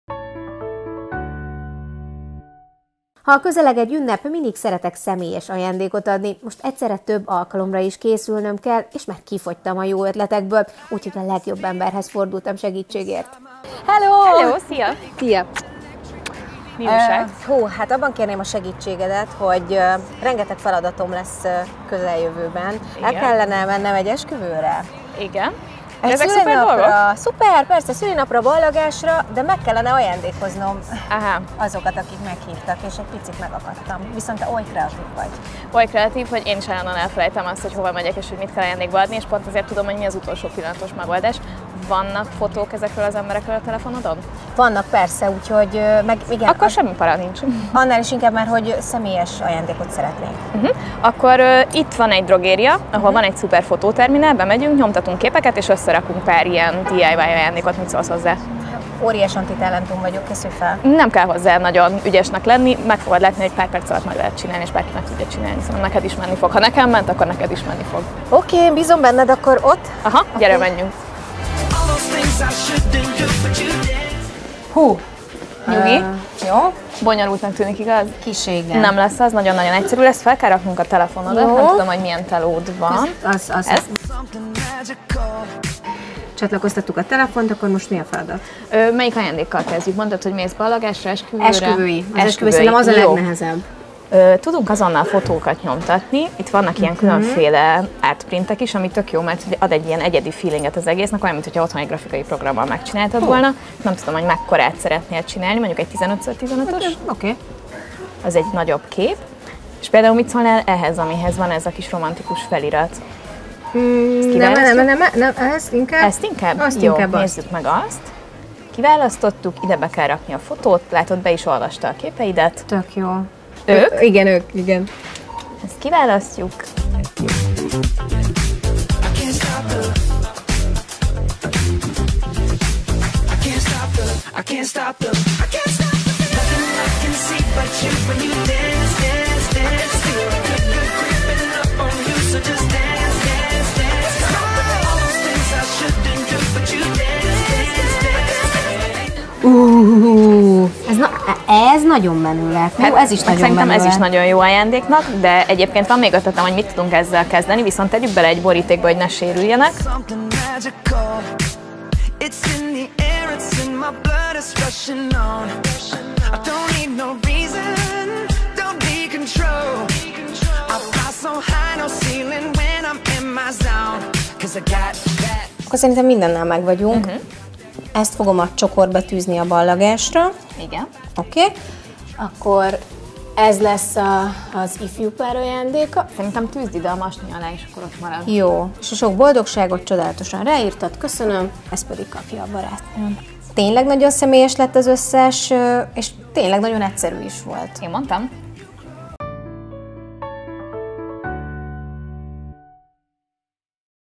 CEWE FOTÓTERMINÁL a Rossmann drogériákban
Egy papírkép sokszor jelent 1-1 kedves emléket fényképen, akár hirtelen kis meglepetésként, ha éppen randira igyekszünk, vagy beurgunk a szüleinkhez! De miért ne lehetne egy lánybúcsún kellék, vagy egy ballagási csokor kiegészítő dekorja? Vagy csak egy friss darab otthon a fotófalunkon? Számtalan ötlet, egy karnyújtásnyira, bármely drogériában - lássuk, mit hoz ki belőle Egy igazi nő:)